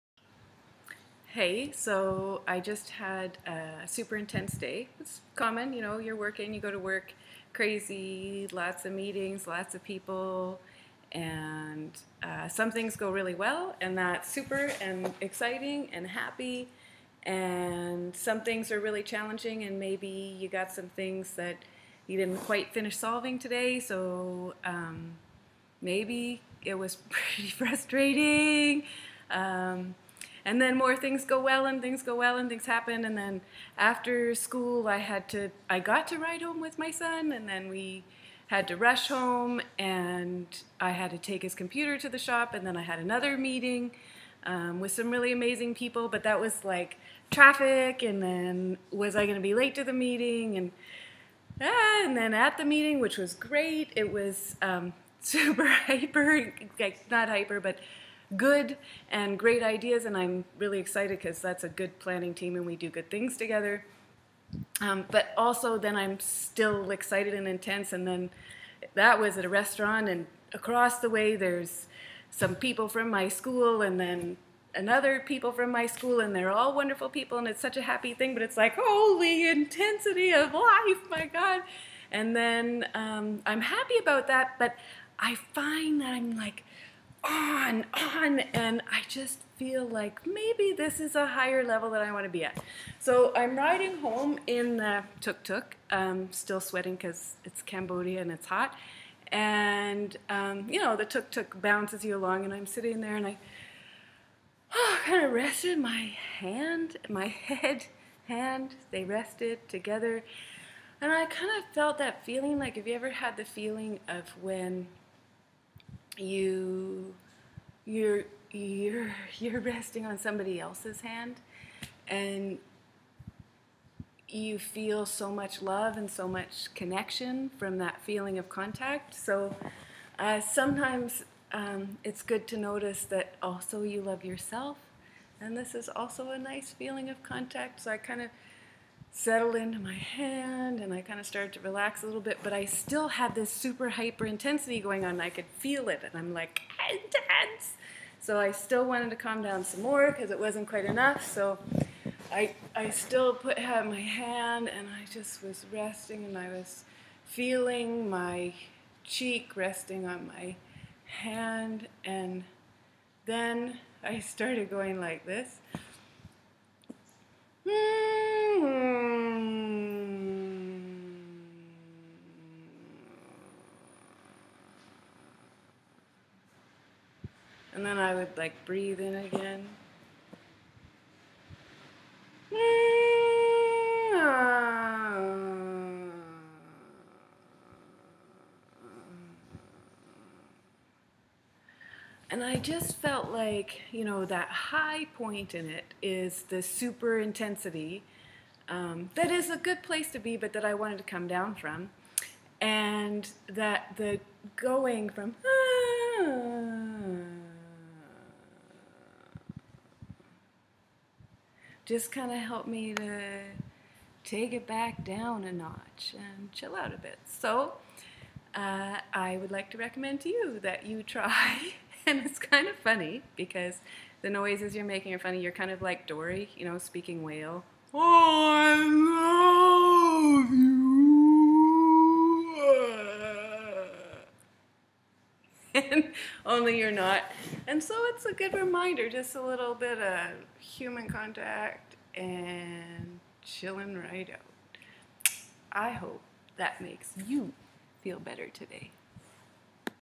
A relaxation technique to help you feel better
Some thoughts and a technique for going from the high intensity of a busy workday to a calm peaceful loving happy chill. I hope it makes you feel better today. xoxoxo